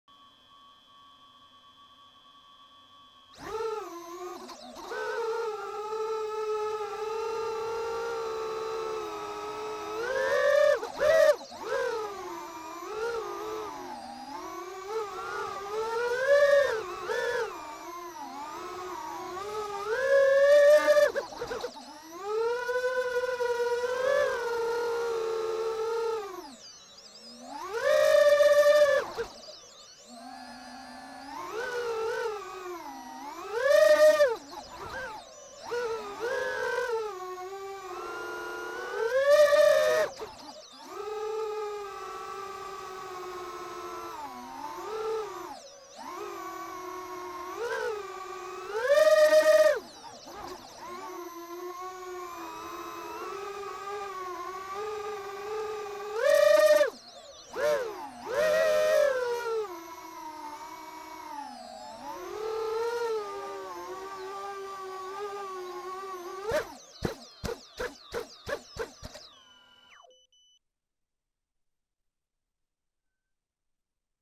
Cinewhoop "freestyle", slowly getting somewhere.
Doing the flying I like to do with a quad not made for doing what I like to be doing but I do it anyways and I enjoy it! Hope my Emax Babyhawk II comes in the mail soon!